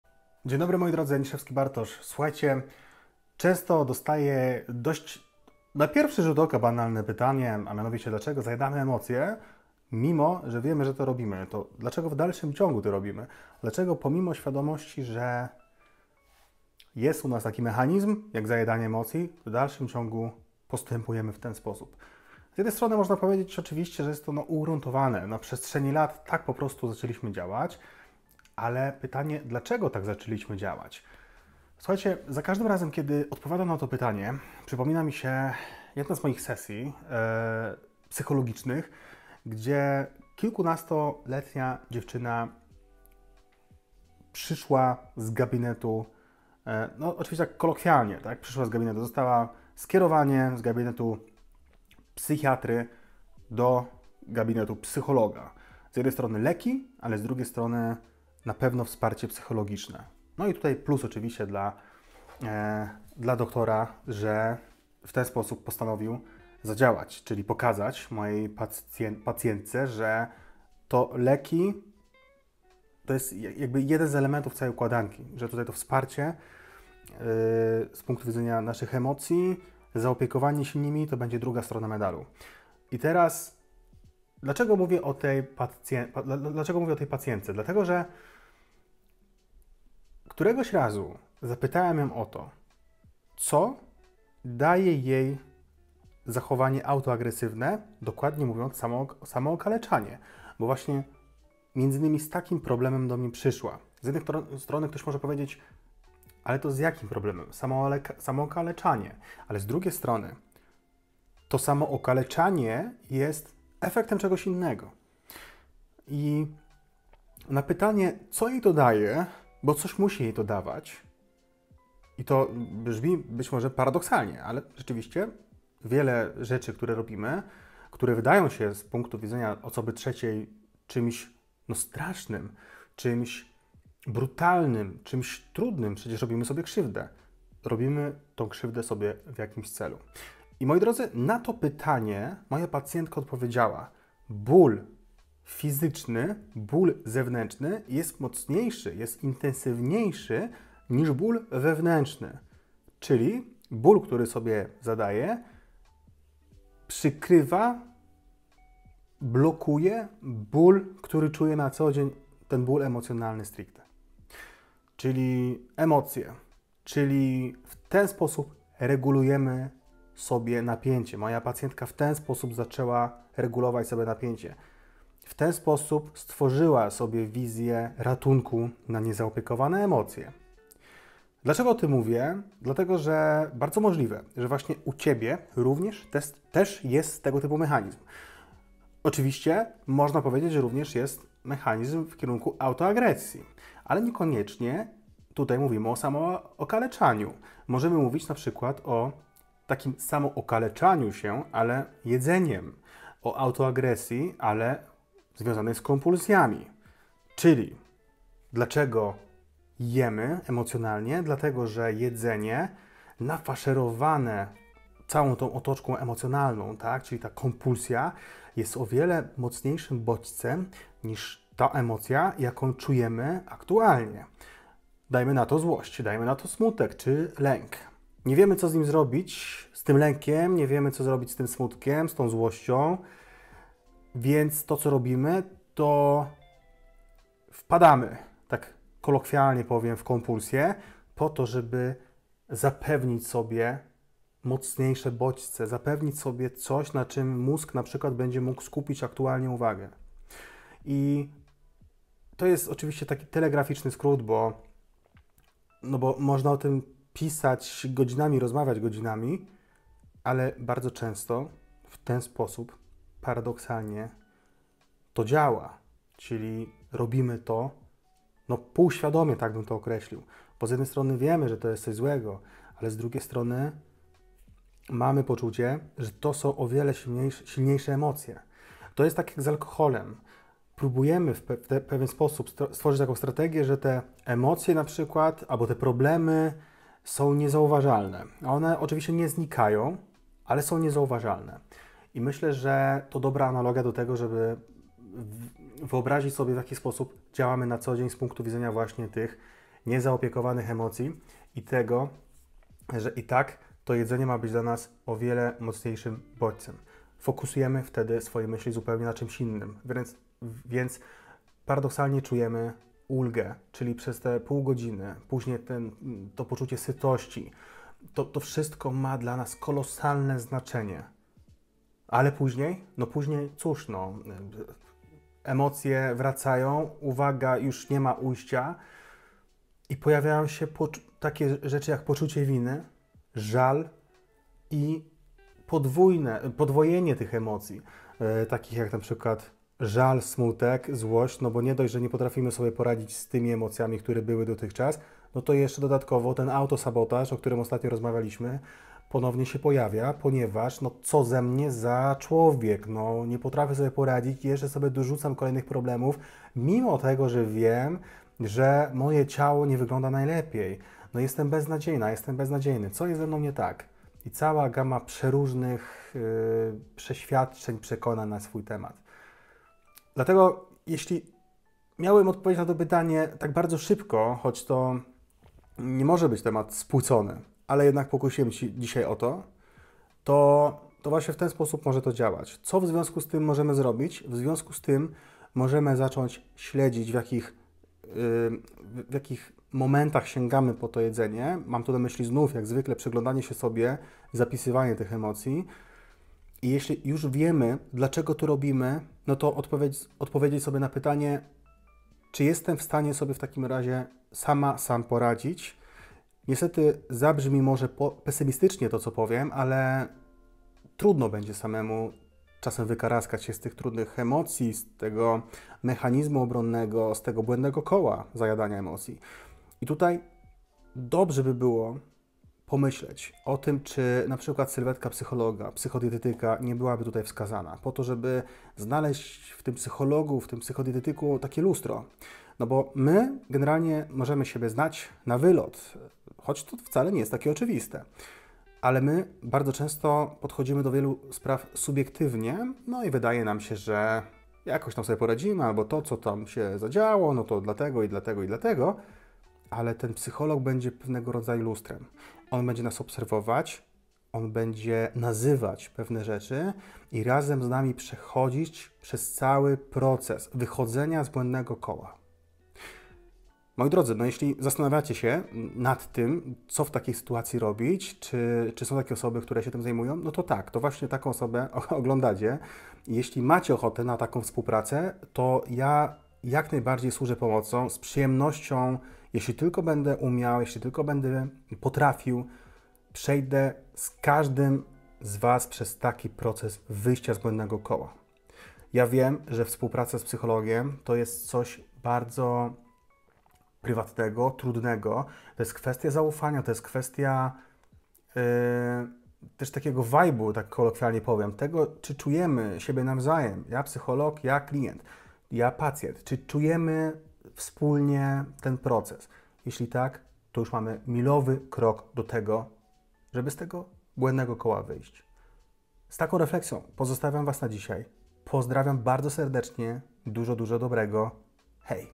DLACZEGO ZAJADAMY EMOCJE?
Czy zdarzyło Ci się sięgnąć po przekąskę, gdy byłeś zestresowany, smutny lub znudzony? Nie jesteś sam! W naszym najnowszym filmie przyjrzymy się zjawisku zajadania emocji. Dowiesz się, dlaczego tak często sięgamy po jedzenie w odpowiedzi na nasze uczucia i dlaczego wbrew pozorom coś nam to daje...?

Pragniesz więcej?